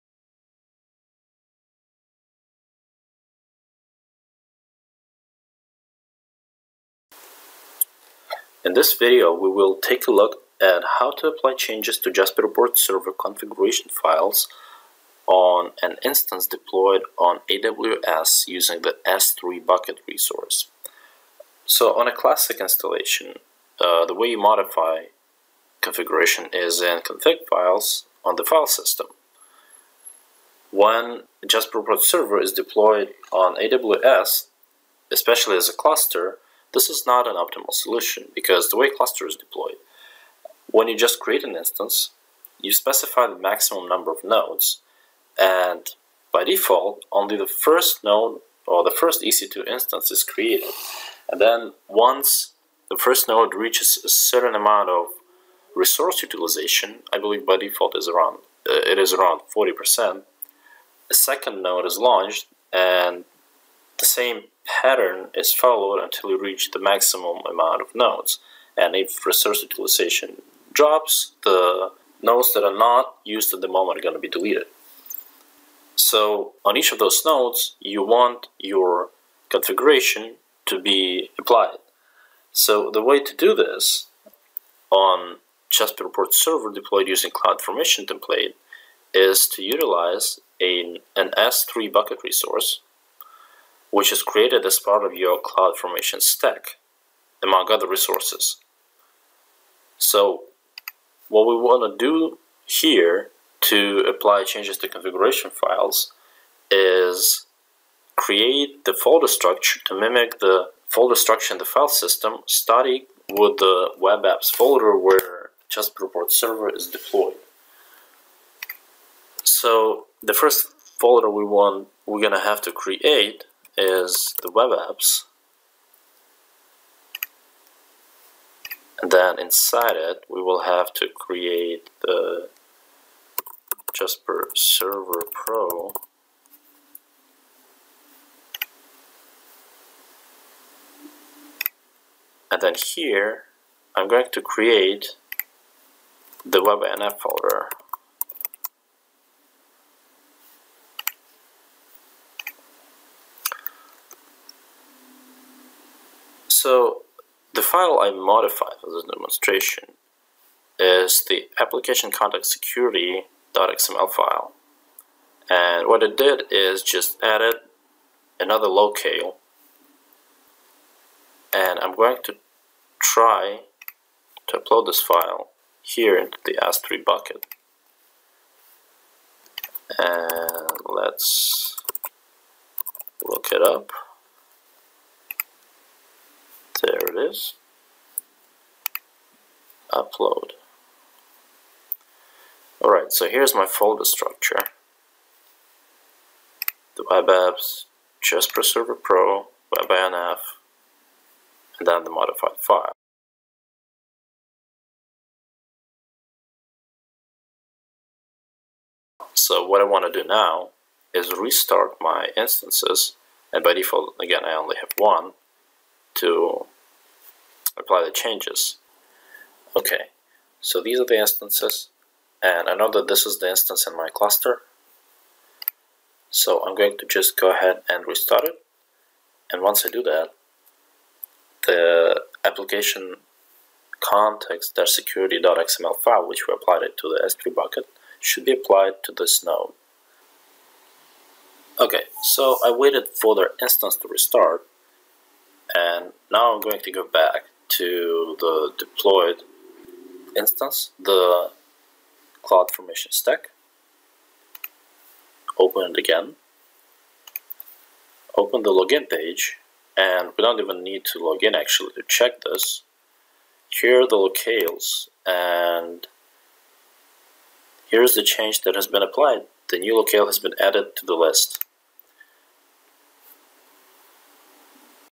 TIBCO Jaspersoft: Jaspersoft for AWS - Editing/Uploading JasperReports Server Configuration Files
This video explains the procedure to edit and upload configuration files to your cloud based instance of Jaspersoft for Amazon Web Services (JasperReports Server deployed on an AMI).

This procedure applies to both SINGLE and CLUSTERED deployments of Jaspersoft/JasperReports Server for AWS. The files edited and uploaded using this method will be deployed to ALL nodes.